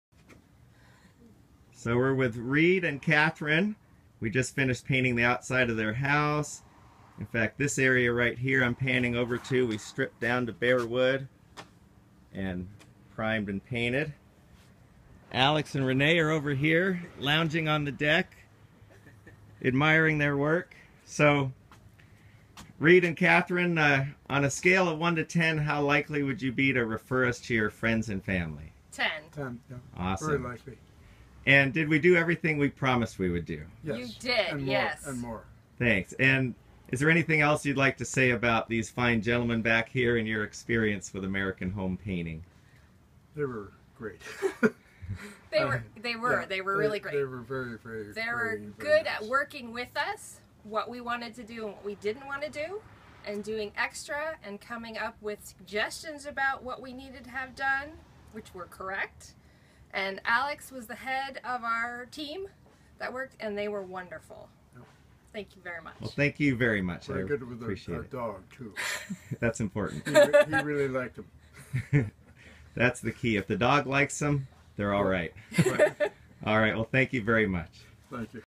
Exterior painting NE Seattle
Exterior painting in the Sandpoint neighborhood of NE Seattle. The job included removing badly peeling paint to bare wood, double priming and using top quality paints.